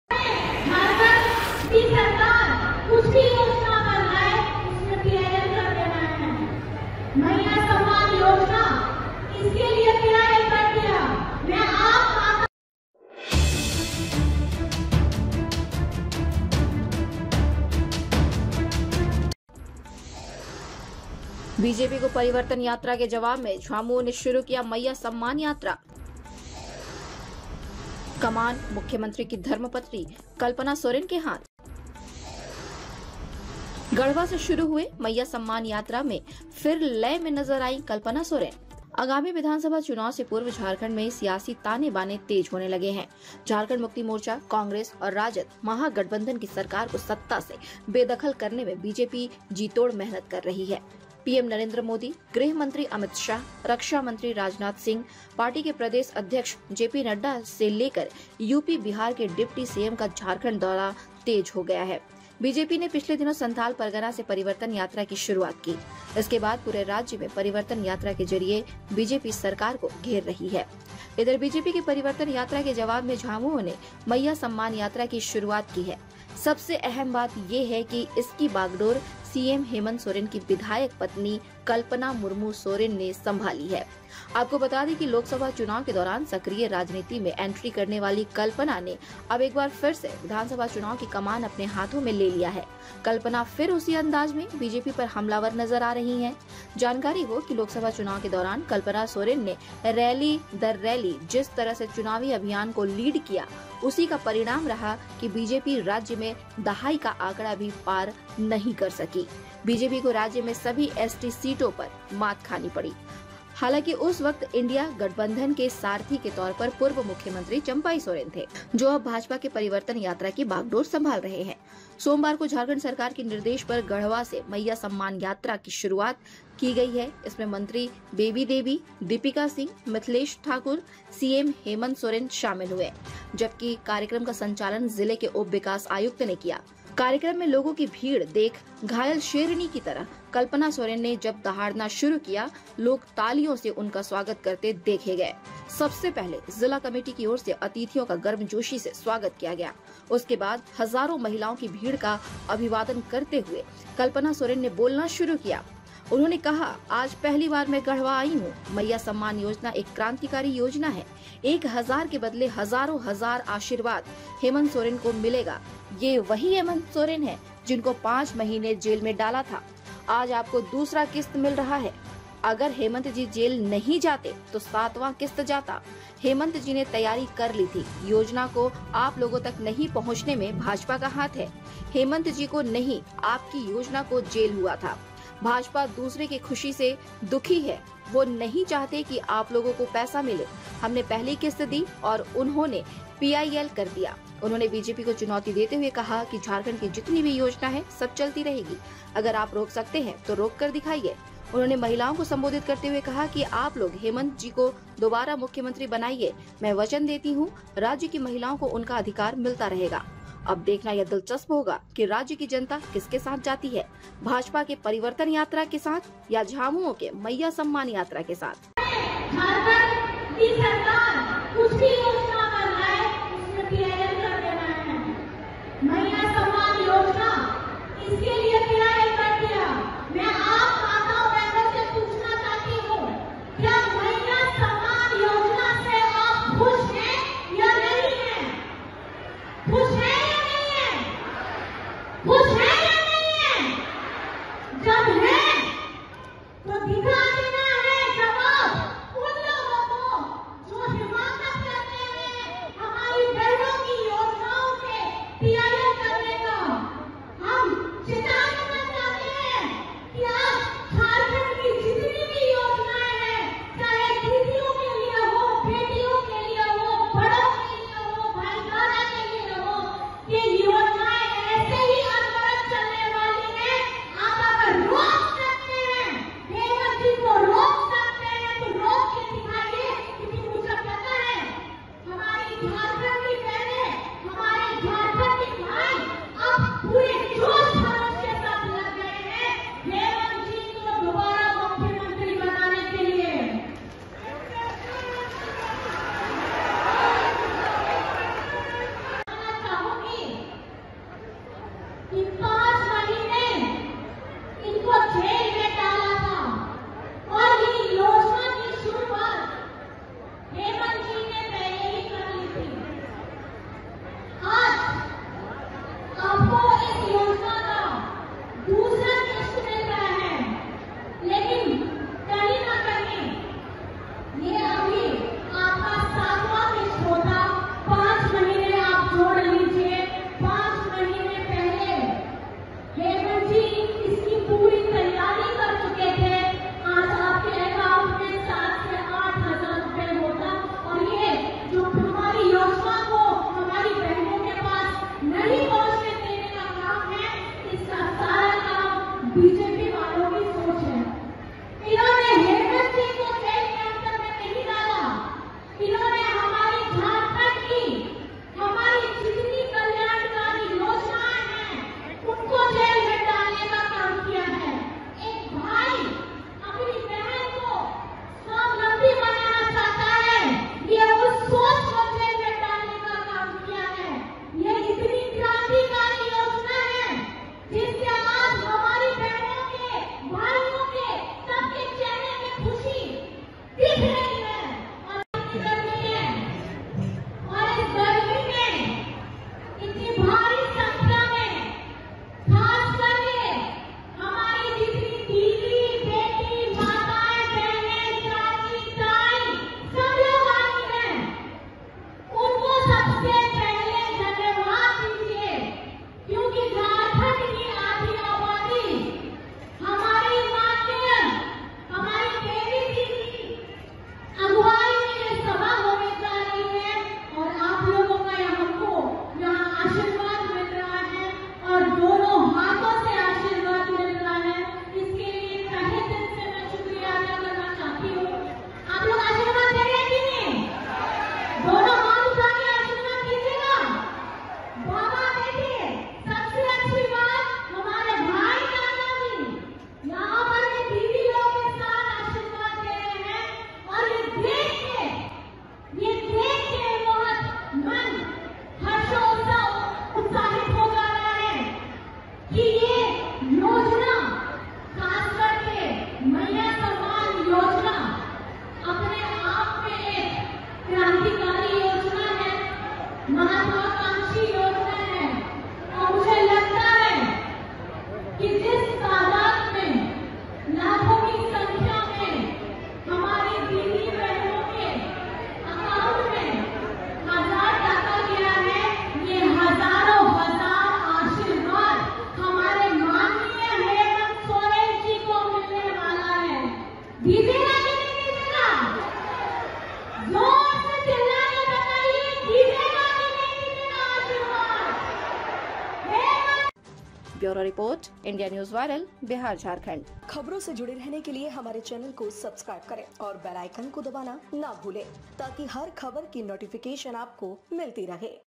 Jharkhand Political Yatra शुरू हुआ झारखंड में राजनीतिक यात्रा, कल्पना के हाथ मंईयां सम्मान यात्रा
आगामी विधानसभा चुनाव से पूर्व झारखंड में सियासी ताने- बाने तेज होने लगे हैं. झारखंड मुक्ति मोर्चा- कांग्रेस और राजद महागठबंधन की सरकार को सत्ता से बेदखल करने में बीजेपी जीतोड़ मेहनत कर रही है. पीएम नरेंद्र मोदी, गृह मंत्री अमित शाह, रक्षा मंत्री राजनाथ सिंह, पार्टी के प्रदेश अध्यक्ष जेपी नड्डा से लेकर यूपी- बिहार के डिप्टी सीएम का झारखंड दौरा तेज हो गया है. बीजेपी ने पिछले दिनों संथाल परगना से परिवर्तन यात्रा की शुरुआत की. उसके बाद पूरे राज्य में परिवर्तन यात्रा के जरिये बीजेपी सरकार को घेर रही है.